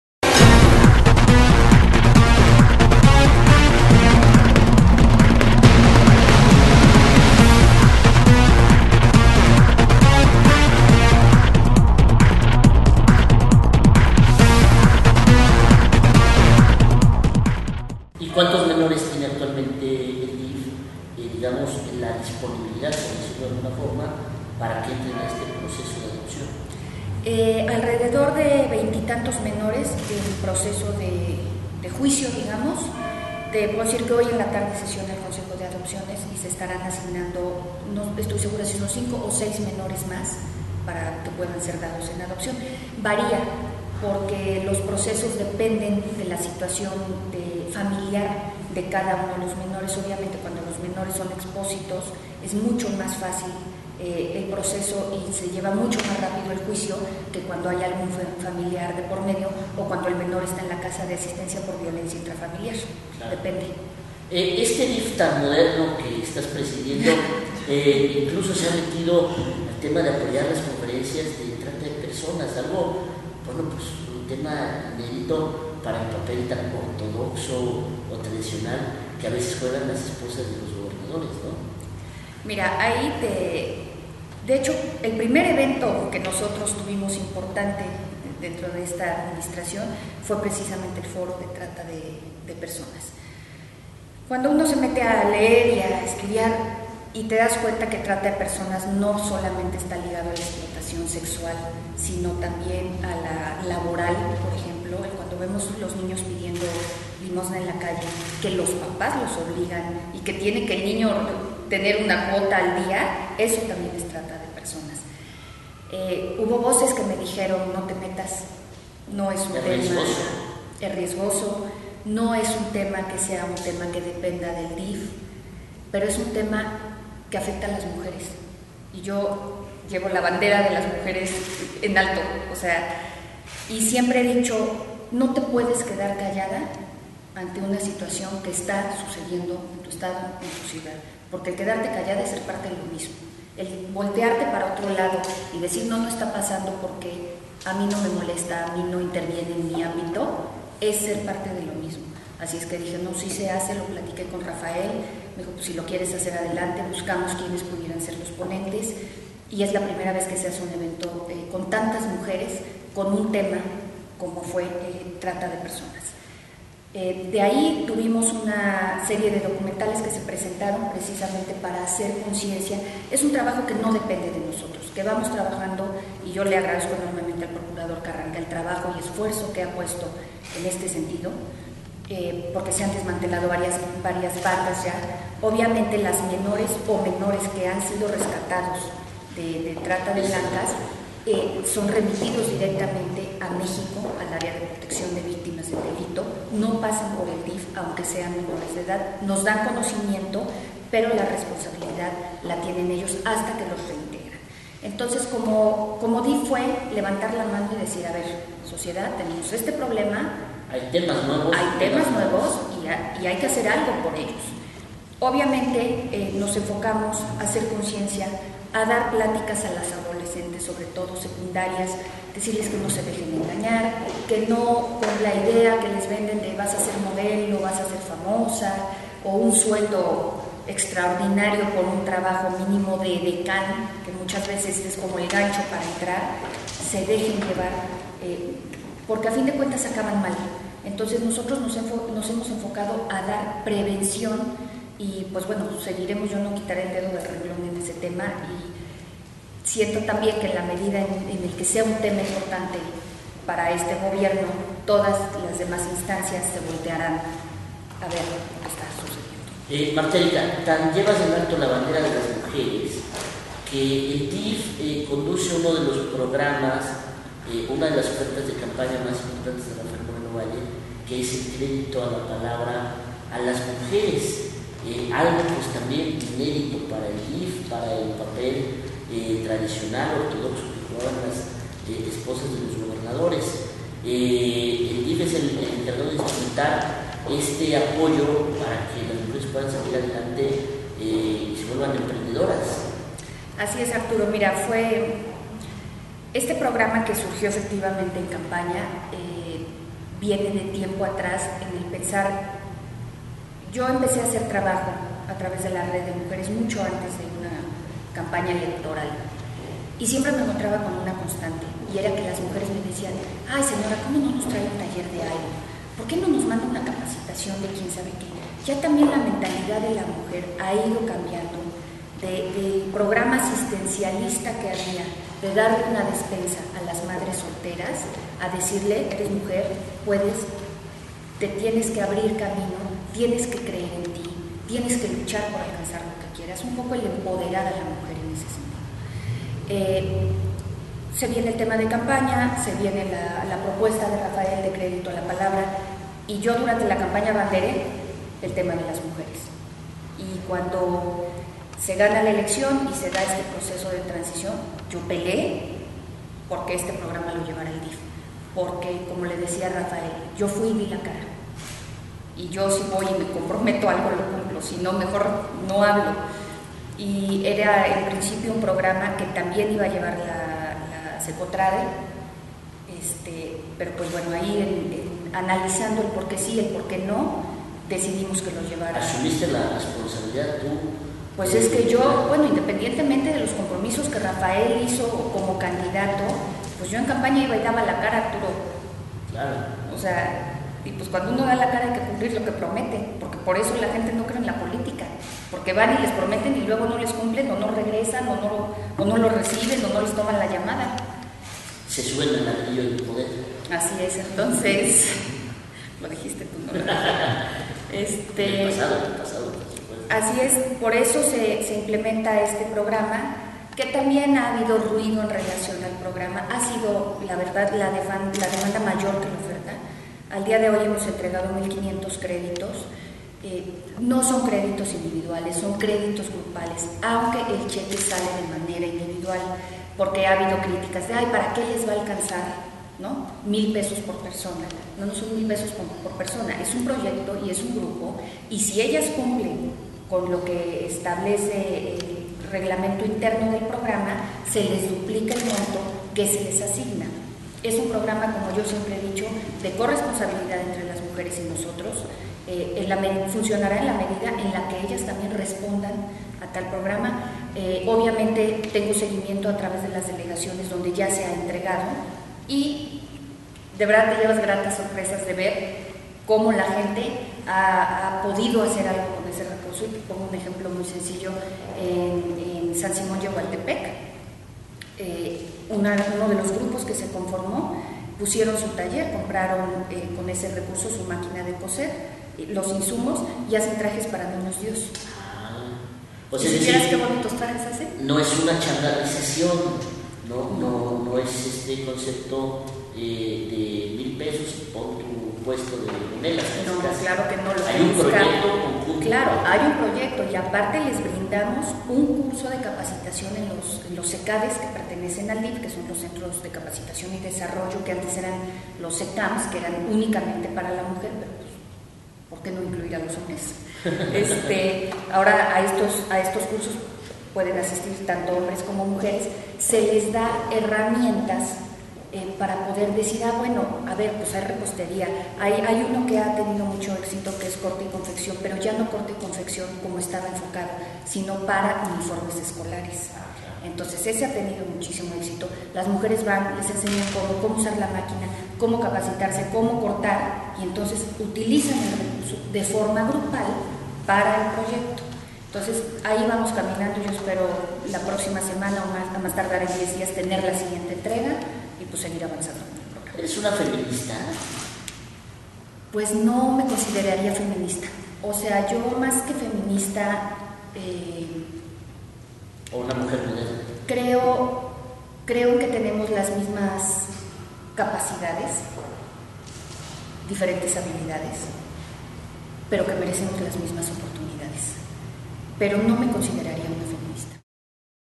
CAMBIO TV: Llevo muy en alto la bandera de las mujeres: Martha Erika
CAMBIO TELEVISIÓN: "Las mujeres tenemos las mismas capacidades, diferentes habilidades, pero merecemos las mismas oportunidades, aunque no me consideraría una feminista". Contundente, una sola frase resume el ideario de Martha Erika Alonso, quien rompe el paradigma del DIF como institución ortodoxa, oxidada, pero también el de la primera dama como compañera sentimental. Y aunque todavía no elige un programa para la posterioridad, tiene claro que los poblanos la recordarán porque "trabajamos mucho, eso te lo puedo garantizar".
El Sistema Estatal DIF, en su nueva época, a la par de los programas tradicionales a cargo del organismo, se atreve a afrontar la trata de personas y agilizar el proceso de adopciones, temas urgentes pero que siguen siendo calificados como tabú.
La esposa del gobernador, en entrevista exclusiva con CAMBIO, se niega a cumplir el rol tradicional de las primeras damas. "Siempre he dicho que no en balde he estudiado, no en balde tengo una maestría para acabar saliendo en una revista donde me pregunten qué tipo de ropa me gusta usar, creo que puedo ir mucho más allá de eso".